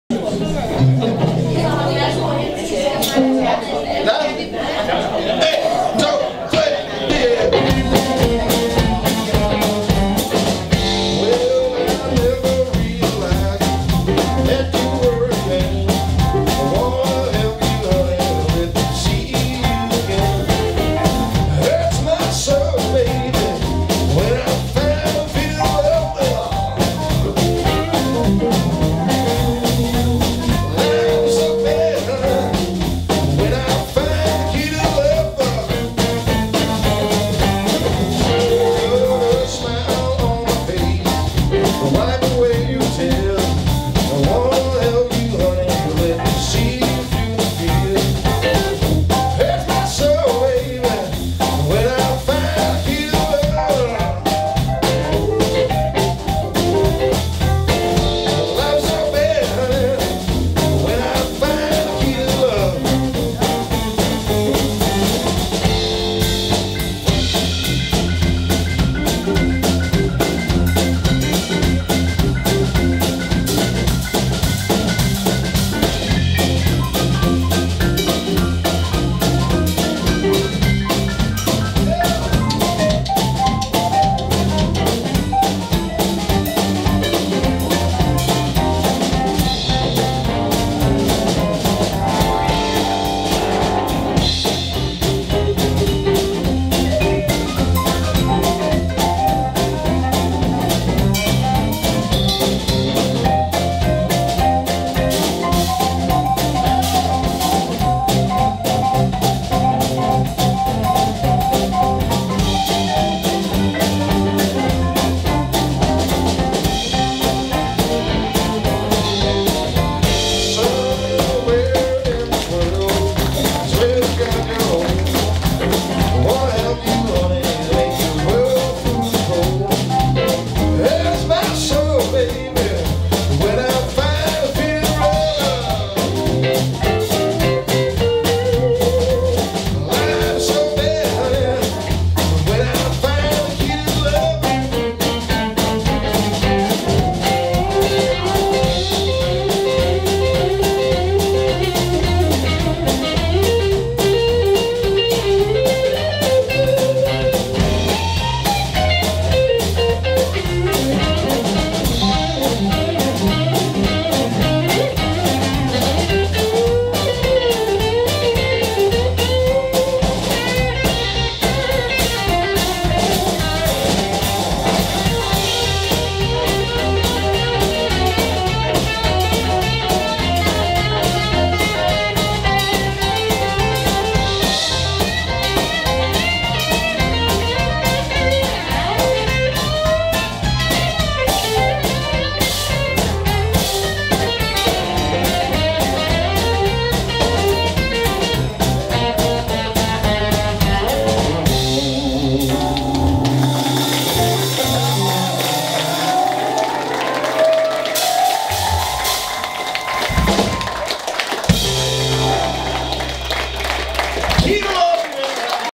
SmallStars Blues Band – 2
Smallstars Blues Jam  d. 5. dec 2019
Vanen tro var hyggen stor på SmallStars med op til flere dygtige sangere og musikere som vi ikke havde hilst på før..
På dette nummer medvirker SmallStars Blues Band